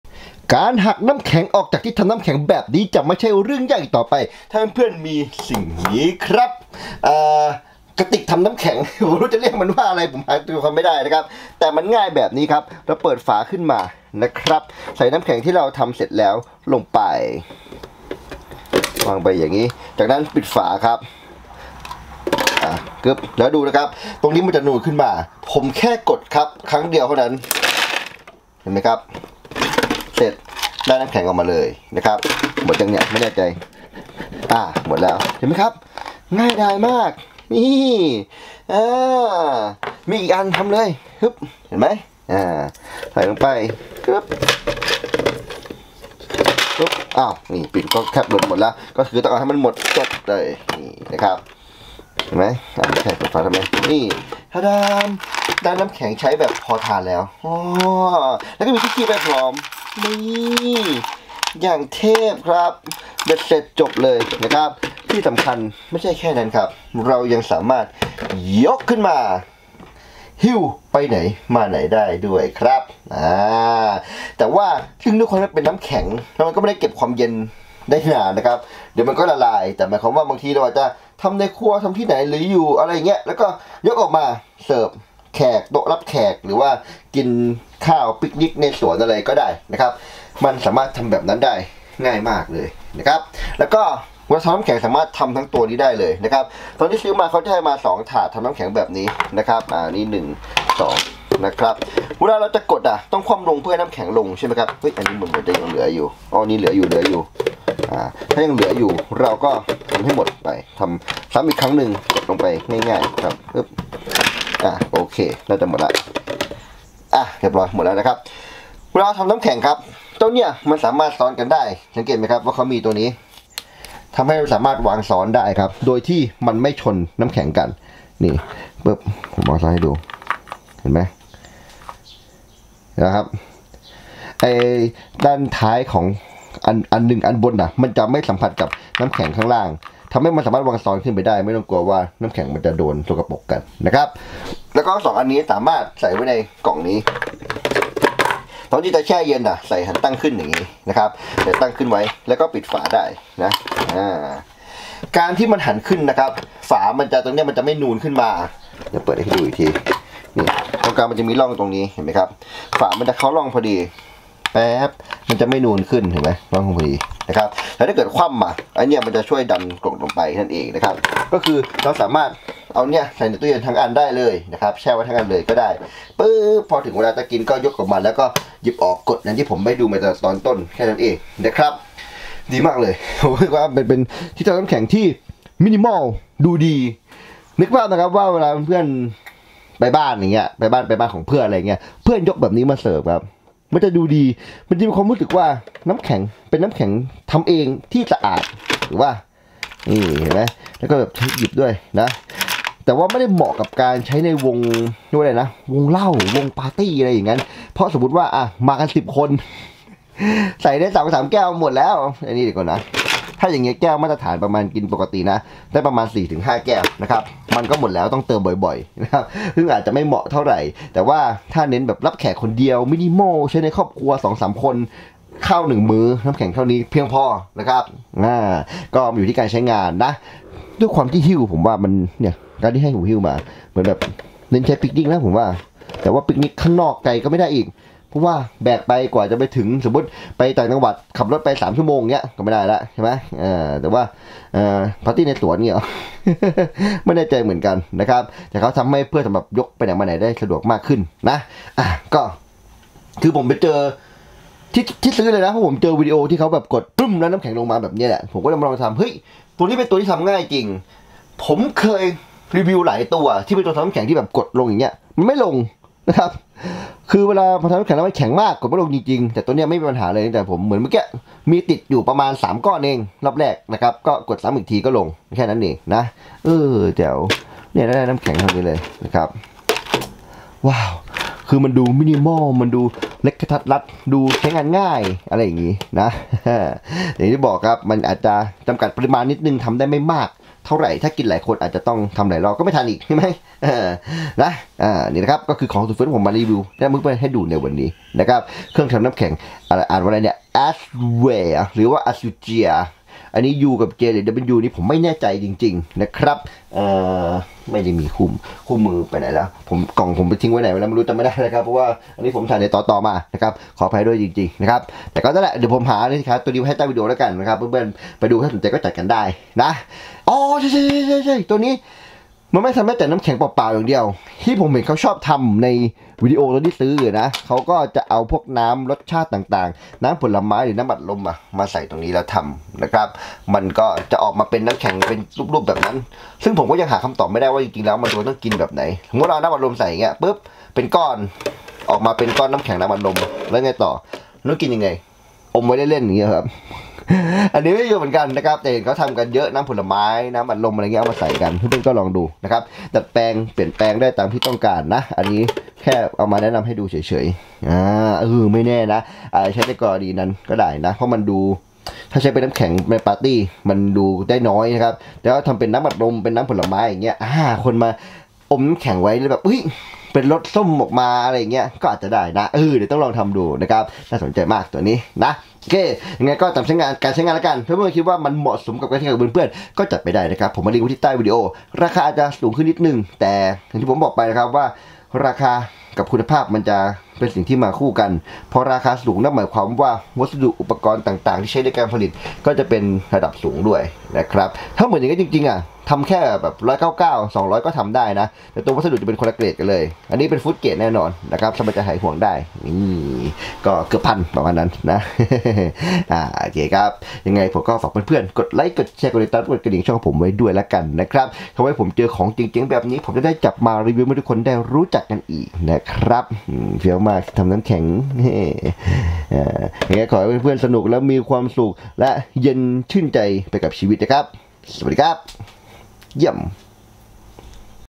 กล่องทำน้ำแข็งด้วยปุ่มเดียว | รีวิวของเจ๋งๆ | เพื่อนซี้ ตัวแสบ 👓
ให้การทำน้ำแข็งเป็นเรื่องง่ายด้วยกล่องนี้สิครับ เป็นทั้งที่ทำน้ำแข็งและกล่องเก็บน้ำแข็งไปในตัว ใช้งานง่านด้วยการกดปุ่มเดียว